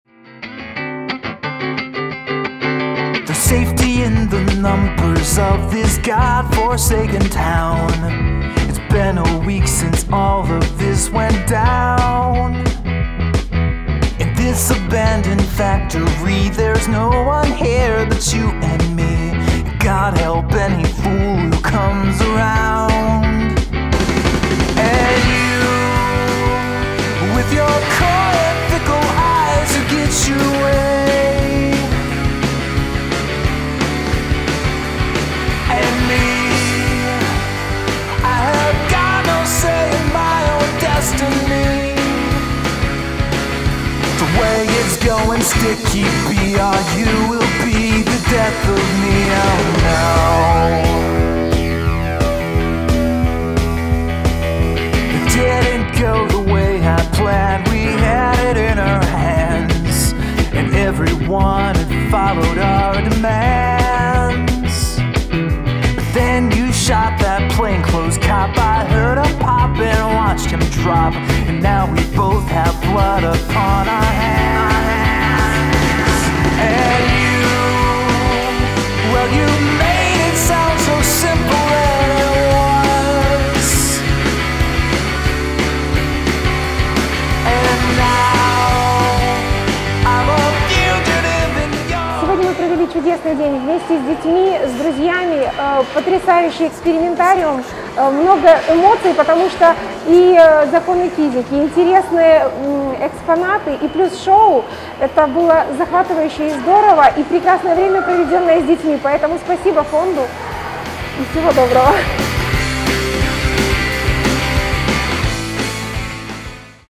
Эксперименты с электричеством на «Шоу Тесла»
Мы продолжаем дарить памятные моменты маленьким украинцам. Чтобы развлечь ребят из многодетной семьи Чеберяк Благотворительный фонд Константина Кондакова пригласил их на познавательное научное «Шоу Тесла» в рамках акции «Ни дня без благих дел!». Партнером Фонда выступил музей занимательных наук «Экспериментариум». Дети узнали множество интересных фактов об  электрическом токе и его свойствах. Остроумные ведущие доступно объяснили маленьким гостям, за счет чего образуются молнии и продемонстрировали, как человек может их...ловить!

Девочкам было очень интересно узнать, почему при расчесывании волосы электризуются - все малыши получили наглядный ответ, благодаря специальной установке.

Физические опыты с электричеством оказались не только интересными, но и зрелищными! У гостей «Экспериментариума» была уникальная возможность познакомиться с таинственными изобретениями Николы Теслы и поглядеть на вспышки огромных электрических дуг, которые звучали, как настоящие музыкальные инструменты. Больше всего детям понравилось, что все экспонаты музея популярной науки и техники можно было потрогать, и почувствовать себя при этом настоящим ученым.

Увлекательные физические эксперименты принесли мальчикам и девочкам из многодетной семьи Чеберяк много радости и новых знаний.

С любовью в сердце мы строим новый мир!